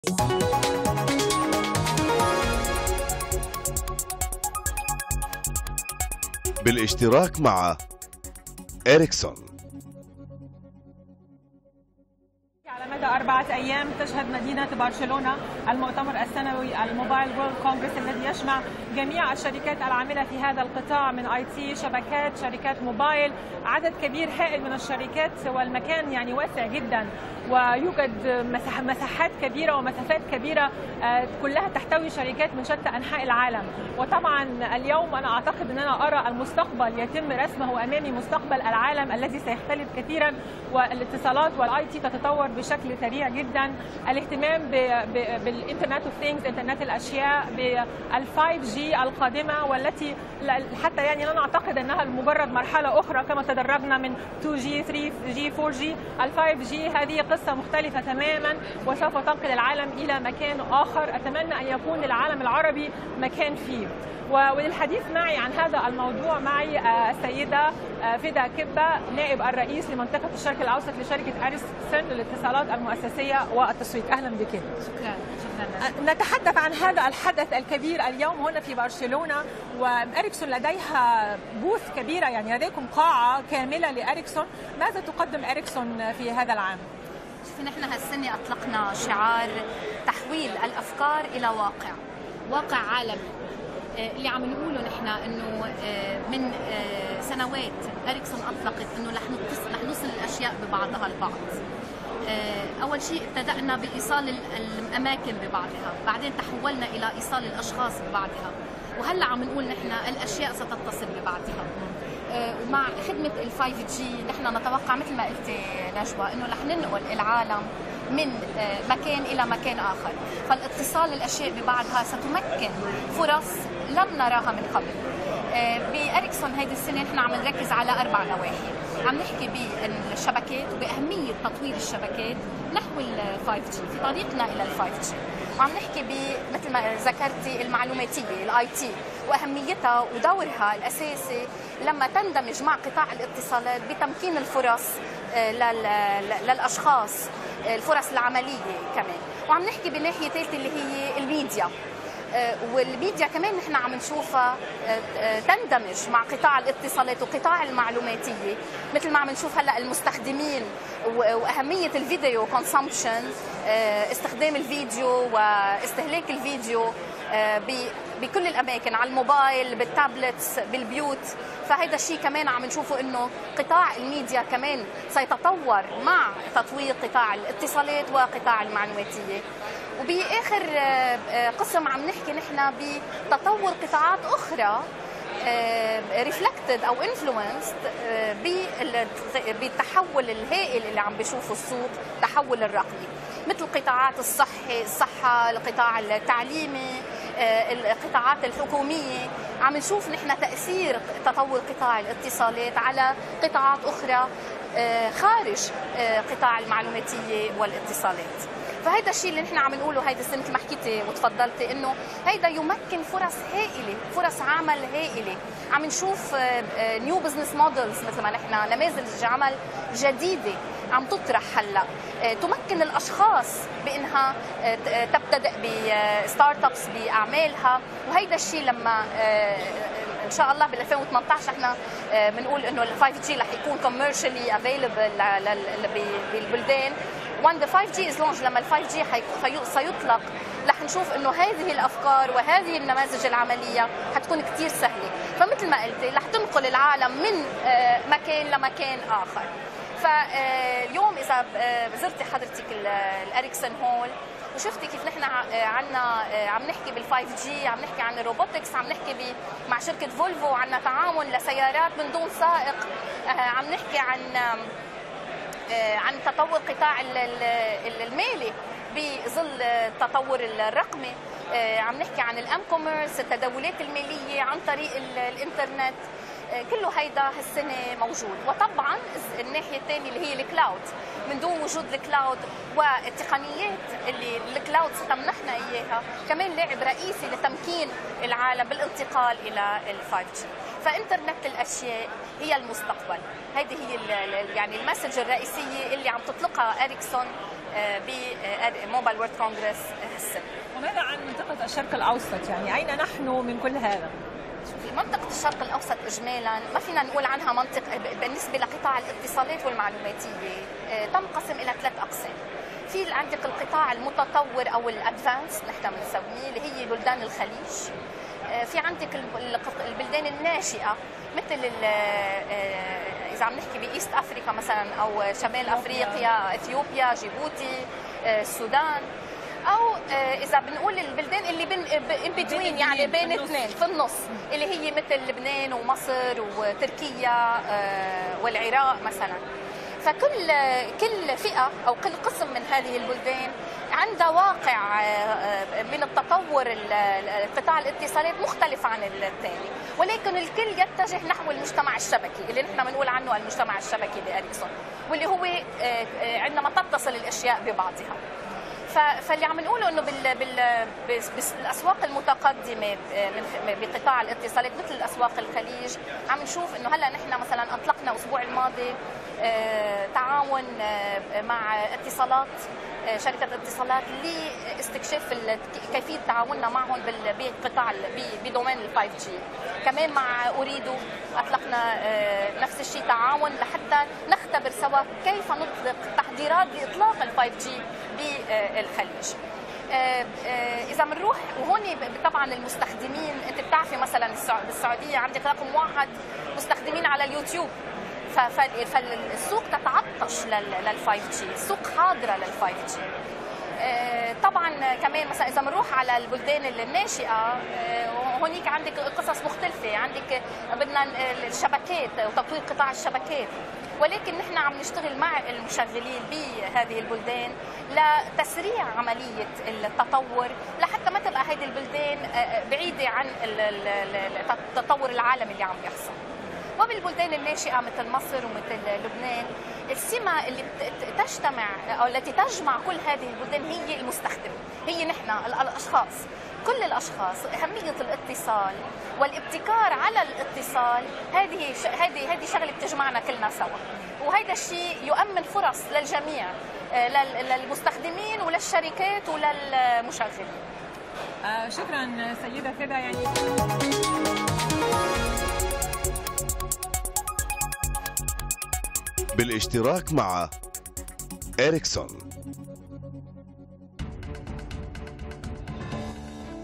"إريكسون" تحضر لاطلاق شبكة 5G خلال سنتين
اعلنت نائب الرئيس ورئيس الإتصالات في "إريكسون" بالشرق الأوسط فيدا كبي عن دراسة مشتركة اجرتها "إريكسون" مع شركتي "اتصالات" الاماراتية ومجموعة "أوريدو" لتحضير إطلاق شبكة 5G في دول الخليج في العام 2018.

وذكرت كبي في حديث خاص مع CNBC عربية على هامش المؤتمر العالمي للأجهزة المحمولة في برشلونة ‎Mobile World Congress‬ ان الشركة اطلقت شعارا جديدا هو "تحويل الافكار الى واقع" بمناسبة اطلاق شبكة الـ 5G الجديدة خلال السنتين القادمتين.

وأكدت كبي ان "إريكسون" ستركز على عدة نواحي اساسية في المعرض وهي اهمية تطوير الشبكات لتصل الى المستوى المقبل الجديد والمتمثل بشبكة 5G، بالاضافة الى اهمية قطاع المعلوماتية وكيفية اندماجها مع قطاع الاتصالات فضلا عن قطاع الاعلام.